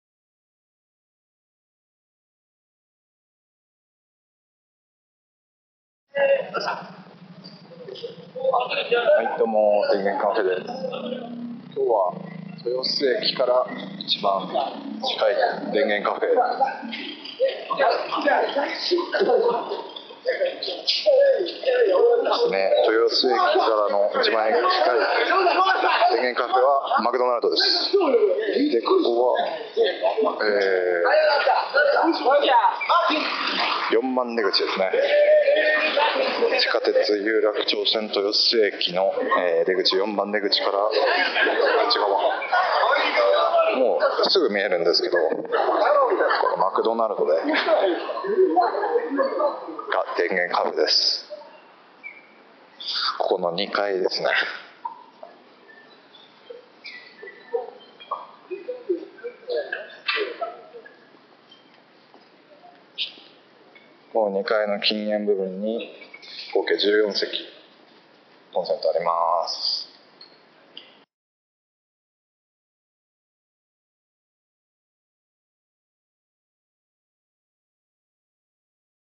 マクドナルド 豊洲駅前店（電源カフェ）
豊洲駅（東京都江東区）付近の電源カフェ：マクドナルド豊洲駅前店への道順を動画でナビします。豊洲駅は東京メトロ有楽町線の駅です。

ネットブック・ノートパソコン・iPhone・iPad・PSP・DS・携帯電話ユーザーのための電源カフェです。

電源カフェって？